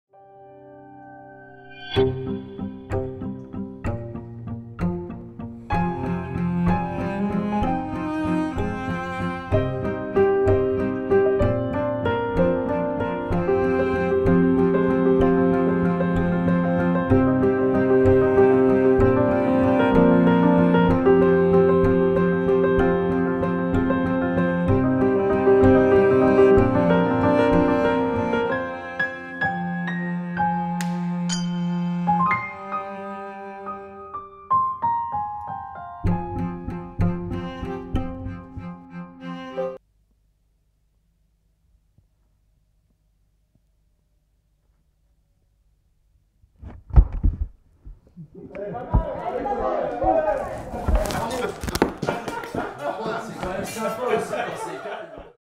Les entretiens de La Rochelle 2019 - Dernier jour
Dernier jour d’entretiens La Rochelle
Se sont mis à table : Sophie Deschamps (SACD), Philippe Rebbot (Un temps de chien), Yannick Choirat (Un homme abîmé), Eléonore Bauer (Vertige)